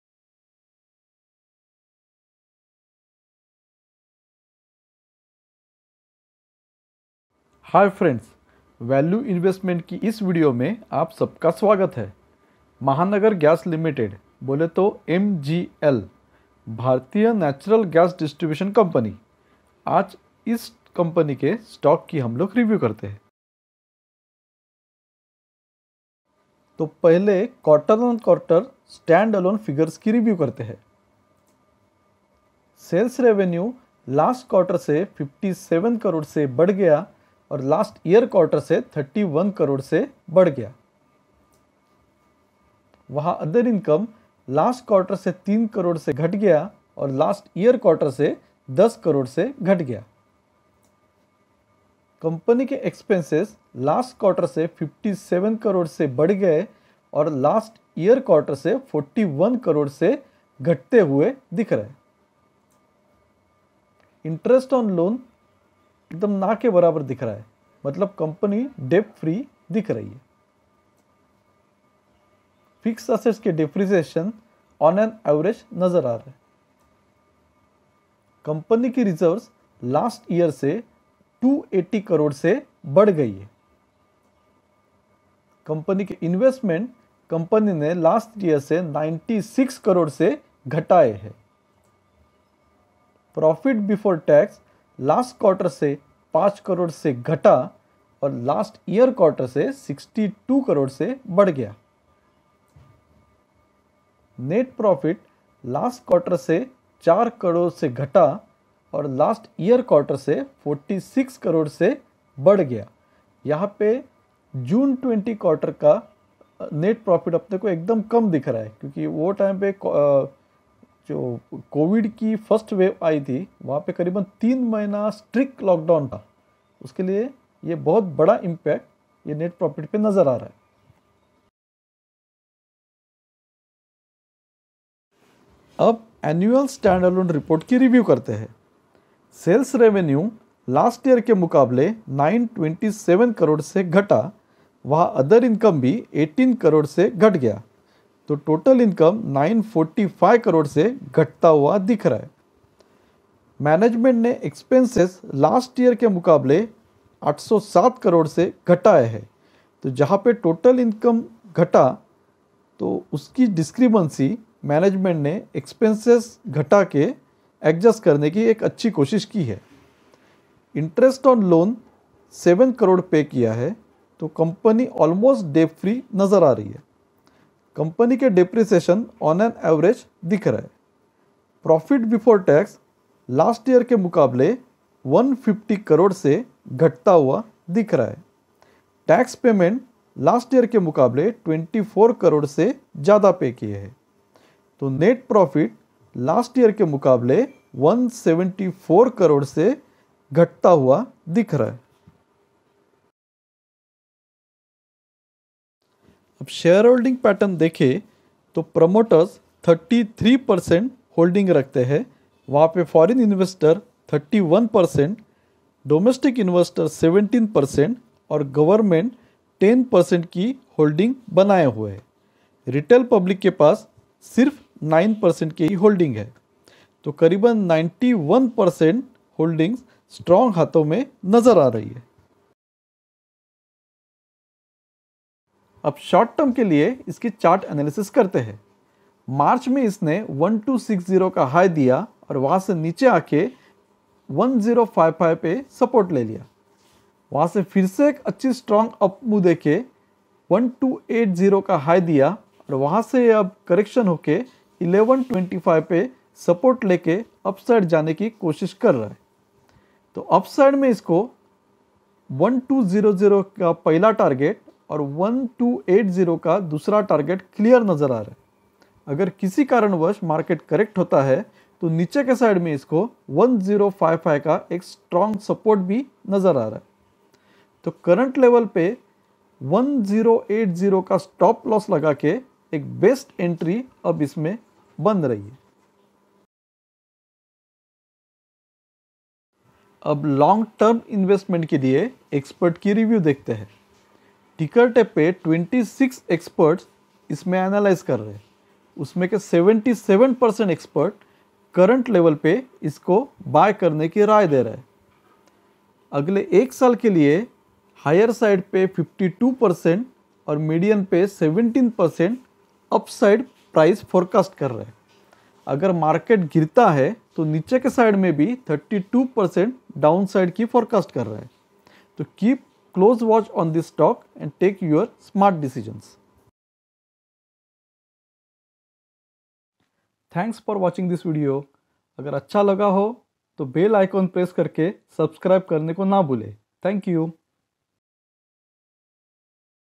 MGL / Mahanagar Gas Ltd क्या अब एक बेस्ट Value Investment बन सकता है ?
MGL / Mahanagar Gas Ltd Stock Analysis.

1. Quarter On Quarter Result Analysis

2. Annual Result Analysis

3. Technical Chart Analysis for Swing Trading

4. Share Holding Pattern

5. Long Term Investment View